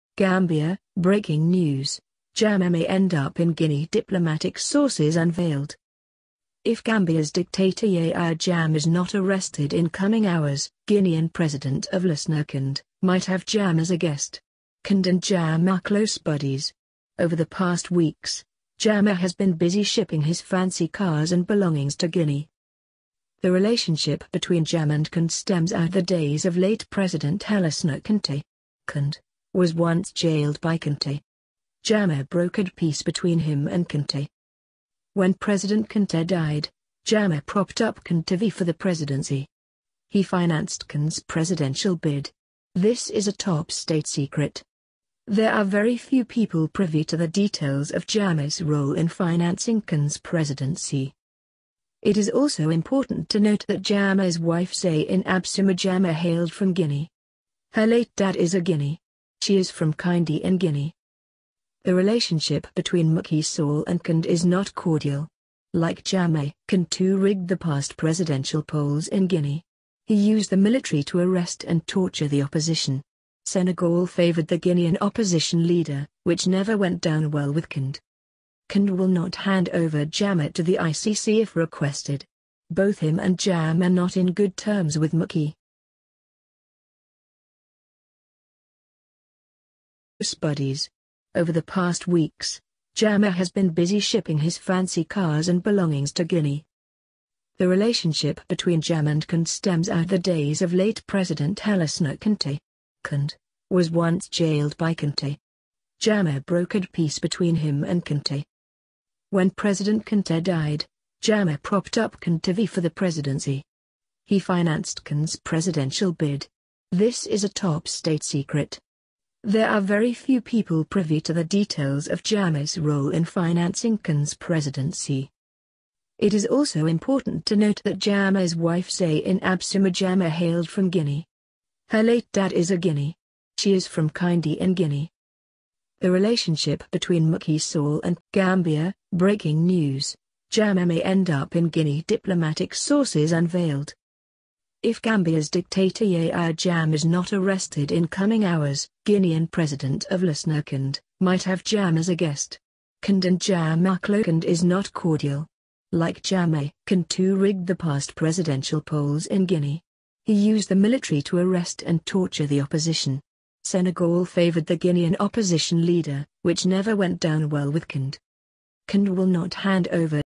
Gambia: Breaking News: Jammeh May End Up In Guinea Diplomatic Sources Unveiled
If Gambia's dictator Yahya Jammeh is not arrested in coming hours, Guinean President of Lasana Conde, might have Jammeh as a guest. Conde and Jammeh .